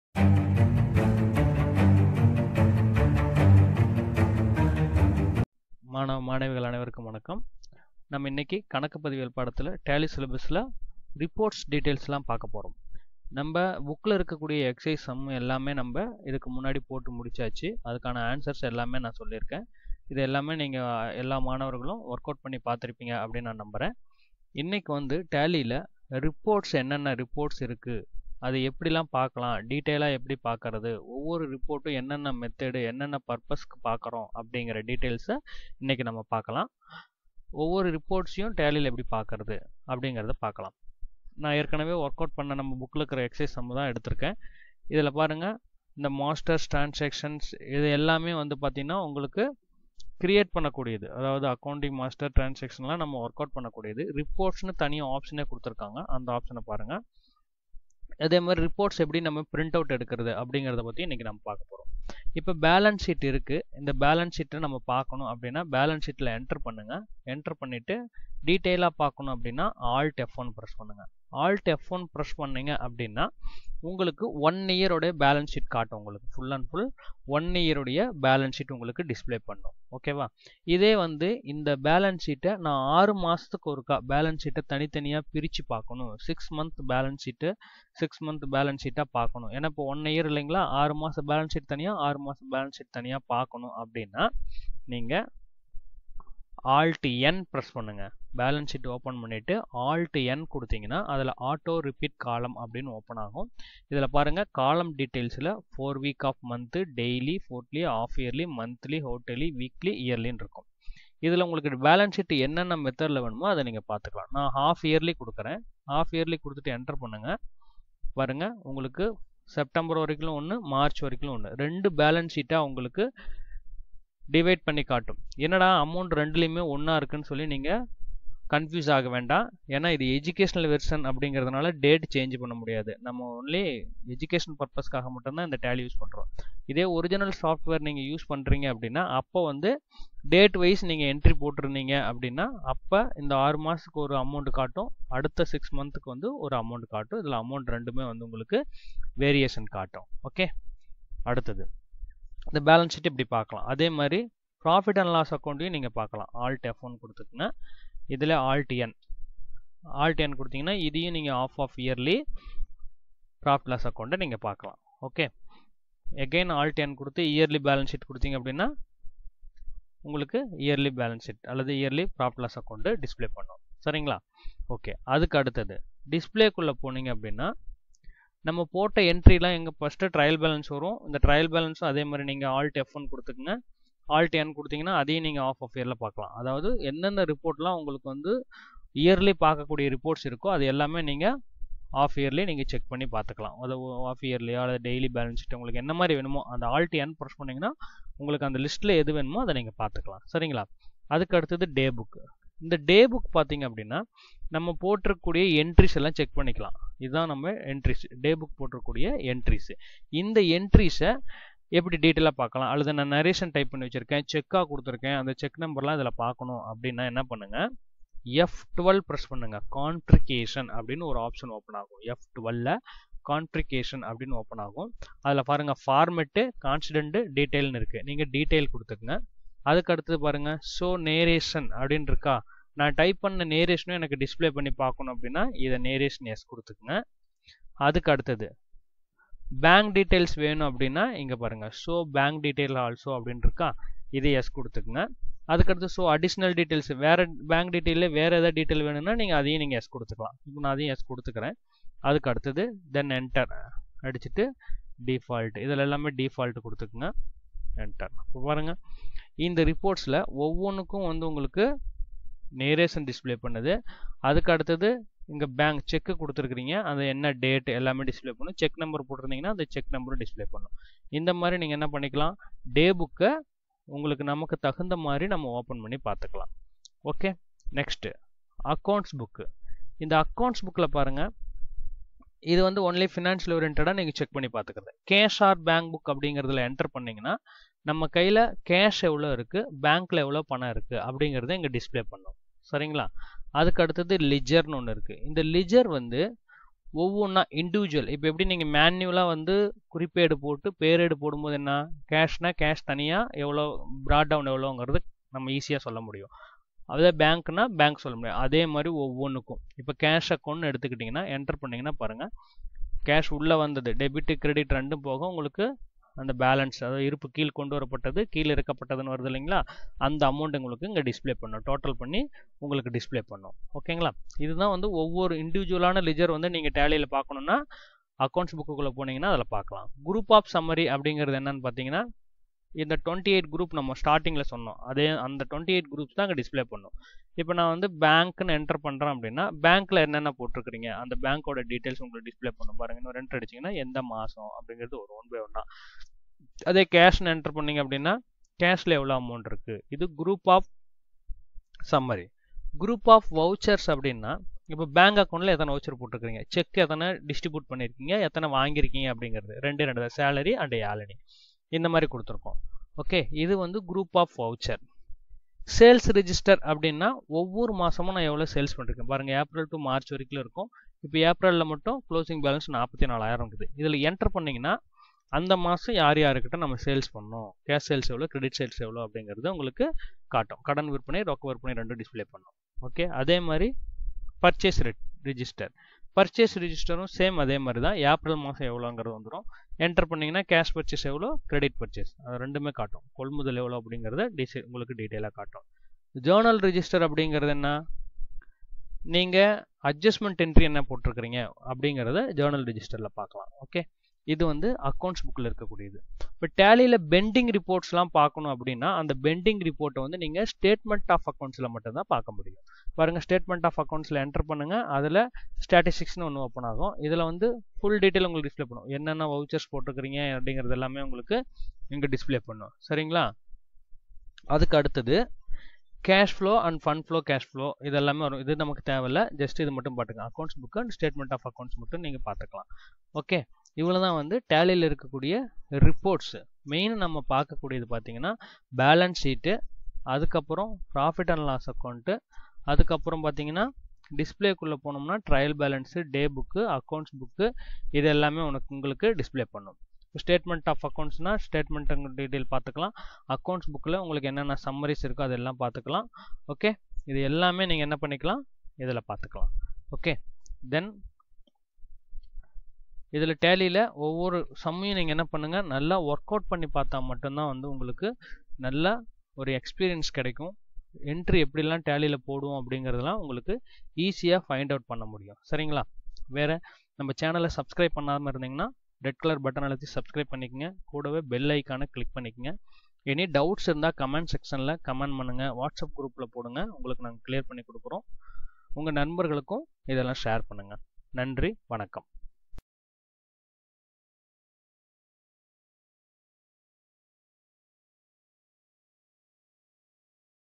12 கணக்குப்பதிவியல்/பாடம் 10. TALLY/PART - 9/தமிழ்@GPTeach
12ம் வகுப்பு கலைப்பிரிவு மாணவர்களுக்கு கணக்குப்பதிவியல் பாடத்தில் டேலி மிகவும் முக்கியமானது.

டேலி பாடத்திலிருந்து கட்டாயம் 2 மதிப்பெண் வினாக்களில் ஒரு கேள்வியும் 3 மதிப்பெண் வினாக்களில் ஒரு கேள்வியும் வர வாய்ப்புள்ளது.

சில நேரங்களில் 5 மதிப்பெண் வினாவிலும் கேட்க வாய்ப்புள்ளது. எனவே மாணவர்கள் டேலி வகுப்புகளை நன்றாக பார்த்து கற்றுக் கொள்ளுமாறு கேட்டுக் கொள்கிறேன்.

வாட்ஸ் அப் குரூப்பில் இணைவதற்கான லிங்க் -